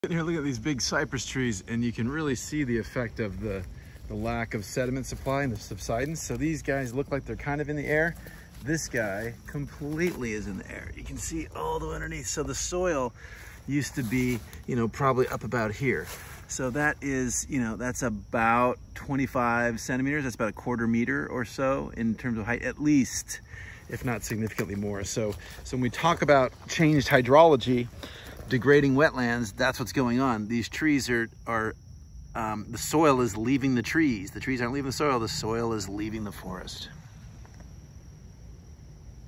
Subsidence and Cypress Root Exposure Woodlands-March 2023
Bald cypress roots and Woodlands Preserve in Belle Chasse, Louisiana.